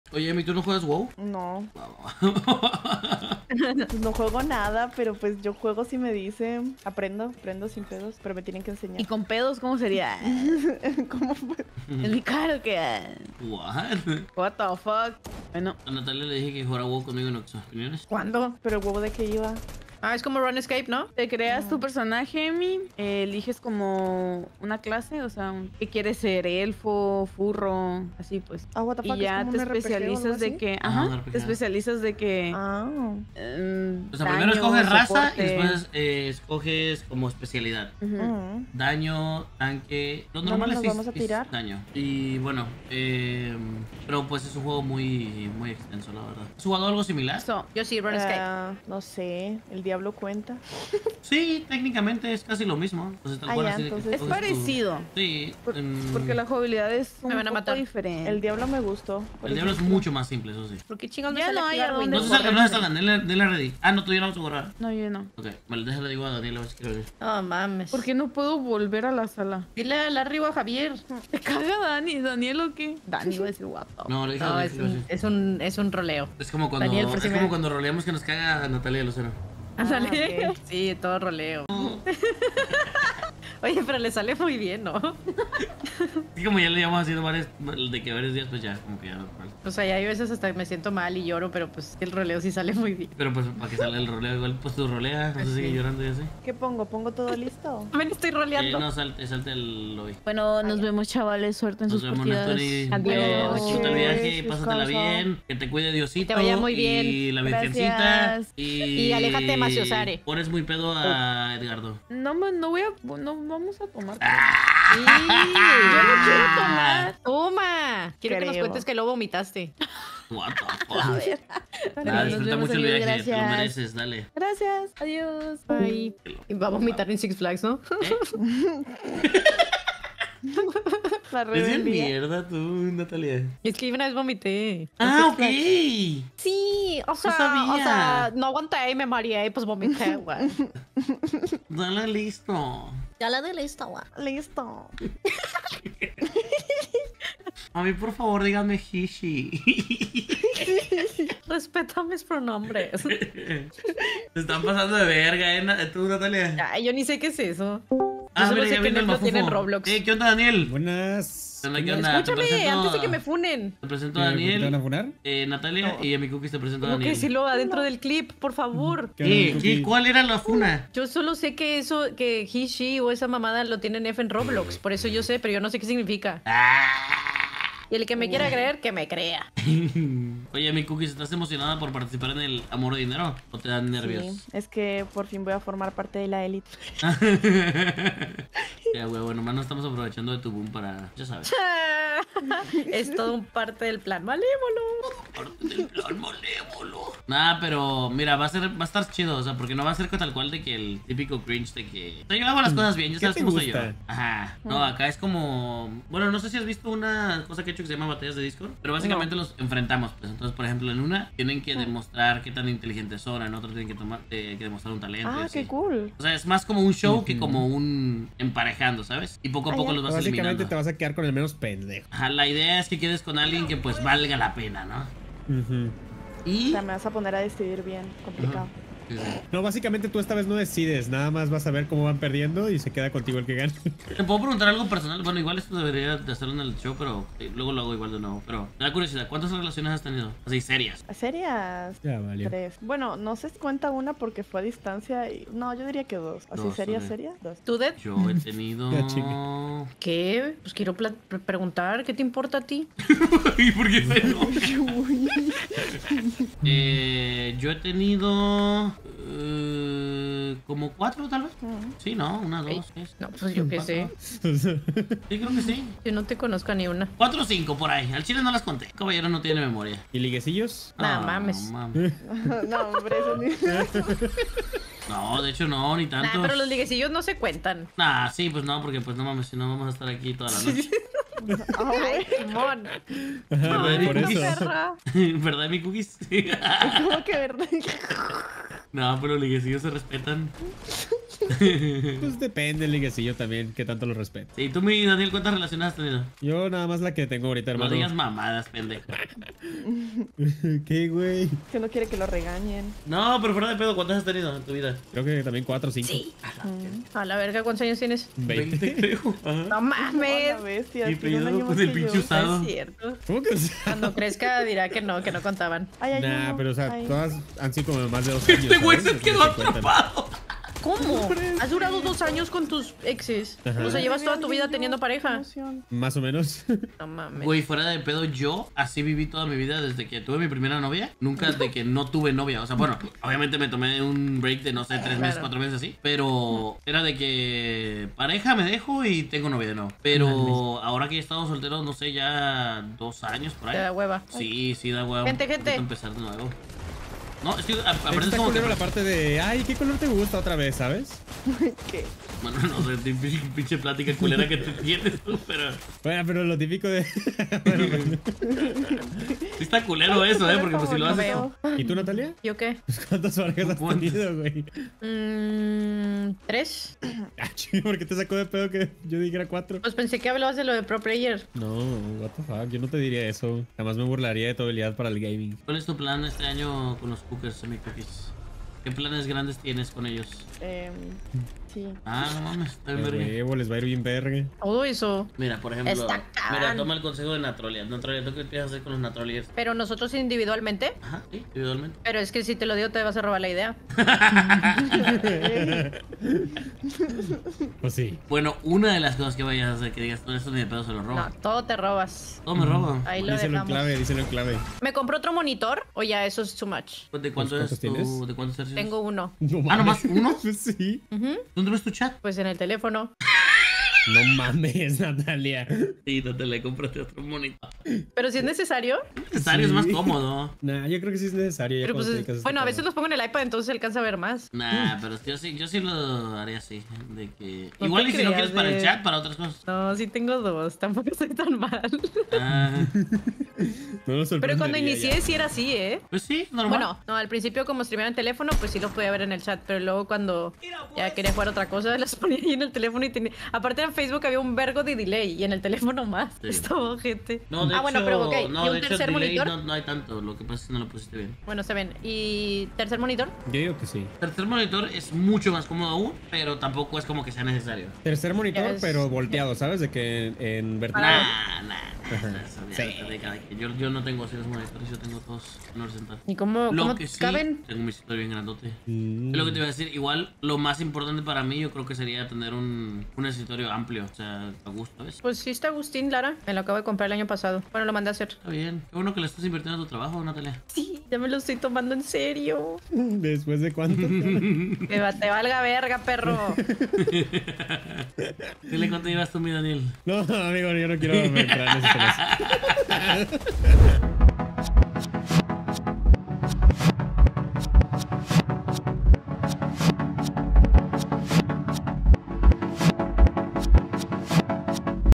ELDED, EMIKUKIS Y NATALIA HABLAN DE RELACIONES
@deDOS  @Emikukis  Y @Nataliamx  HABLAN DE RELACIONES

en llamada @Nephtunie @Daniel_Zoom